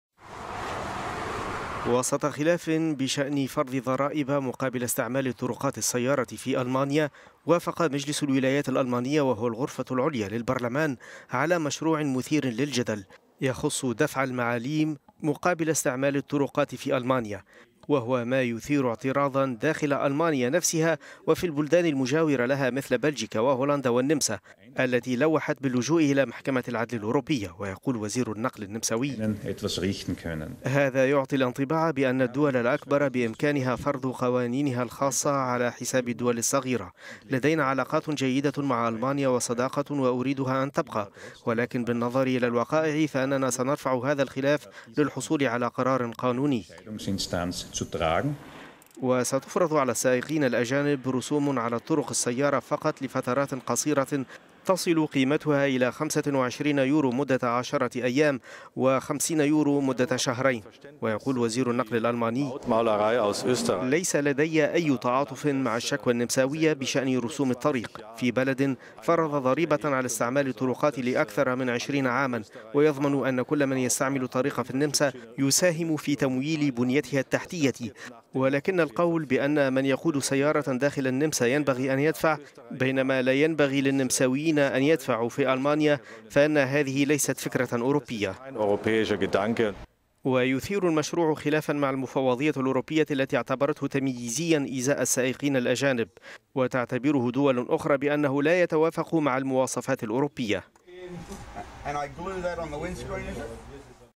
فرض ضرائب في ألمانيا على مستعملي الطرقات السيارة الأجانب
وافق مجلس الولايات الألمانية وهو الغرفة العليا للبرلمان على مشروع مثير للجدل، يفرض ضرائب مقابل استعمال الطرقات السيارة في ألمانيا، وهو ما يعترض عليه أناس داخل ألمانيا وفي البلدان المجاورة لها مثل بلجيكا وهولندا والنمسا، التي اعتبرت موافقة المفوضية الأوروبية عليه بمثابة فضيحة، قائلة إنها ستتجه إلى محكمة العدل الأوروبية للبت فيه

ويقول وزير النقل النمساوي يورغ ليشتفيرد: هذا يعطي الانطباع بأن الدول الأكبر بإمكانها فرض قوانينها الخاصة على حساب الدول الصغيرة. لدينا علاقات جيدة مع ألمانيا وصداقة، وأري…